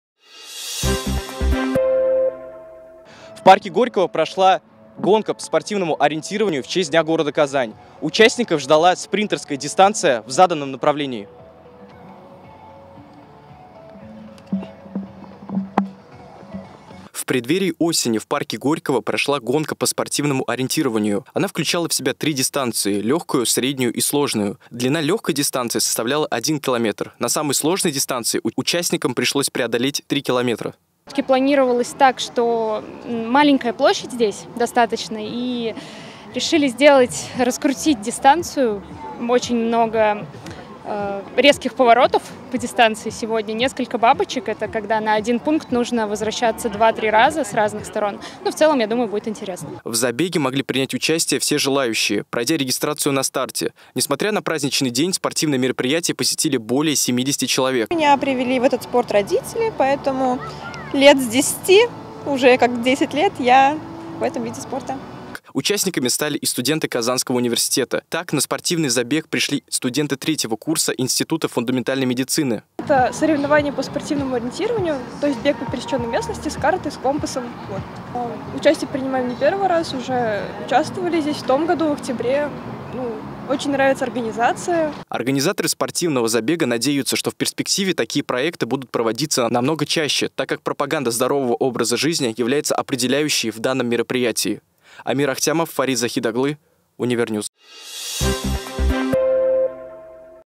ЗАБЕГ В ПАРКЕ ГОРЬКОГО
В парке Горького прошел спринтерский забег. О том, как это было, смотрите в сюжете.